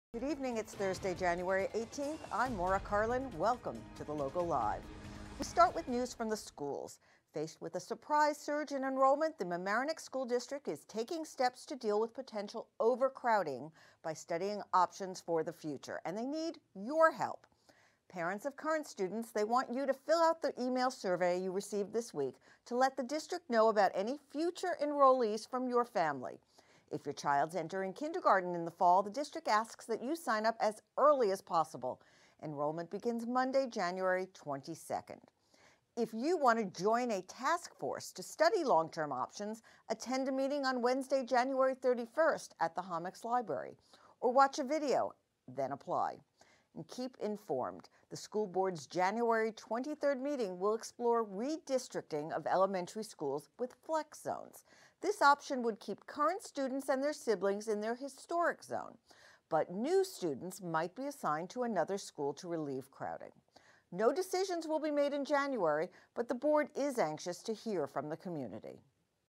Mamaroneck School Overcrowding
There has been an increase in student enrollment in our community. The Mamaroneck School District has been researching options to deal with the situation. And they need your help. The school district is urging parents of current students to fill out the survey that was sent via email last week to let the district know if you have any children will be enrolled in the near future. If your child is entering kindergarten this coming fall, the district encourages you to enroll your child as soon as possible. Enrollment begins on Monday, January 22nd. If you’re interested in joining the task force that is studying long-term options, you can attend the meeting on Wednesday, January 31st at the Hommocks Library or you can watch a video. The topic of the January 23rd meeting of the Mamaroneck School board will analyze the redistricting of elementary schools with flex zones. This option would allow current students and their siblings in their historic zone, but new students could be assigned to another school to alleviate classroom crowding. Decisions will not be made at this time, but the board is curious to hear from the community.